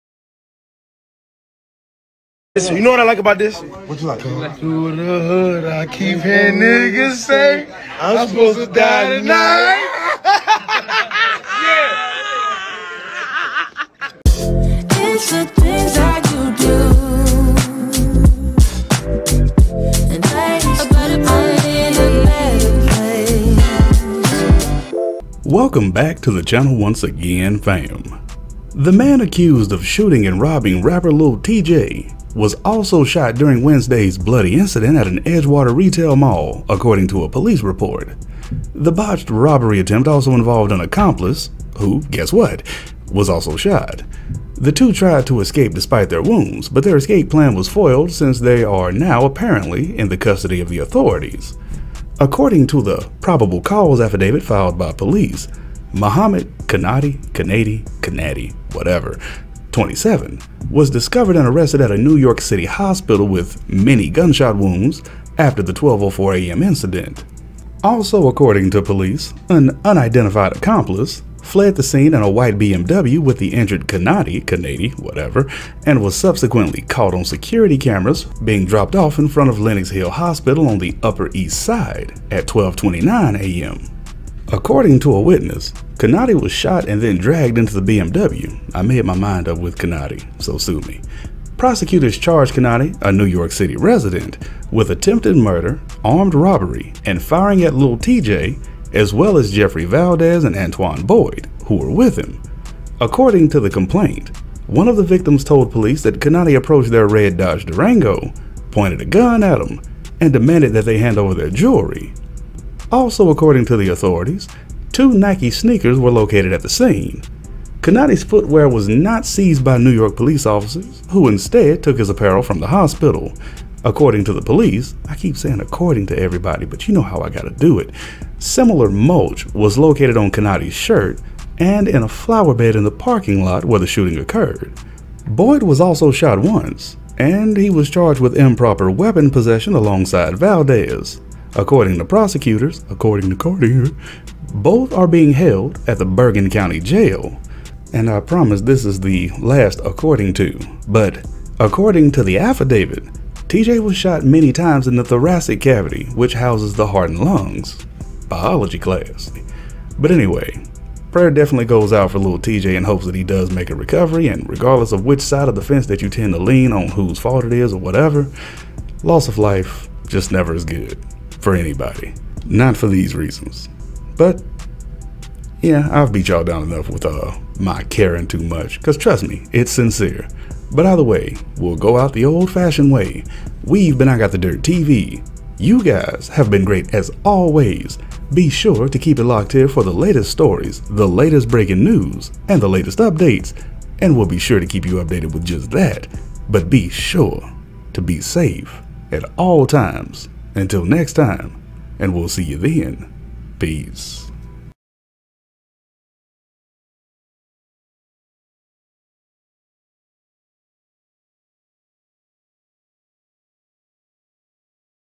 50 Cent Links With Lil Tjay After Being Shot More Times Than Him. The New 50?
50 Cent Links With Lil Tjay After Being Shot More Times Than Him. The New 50?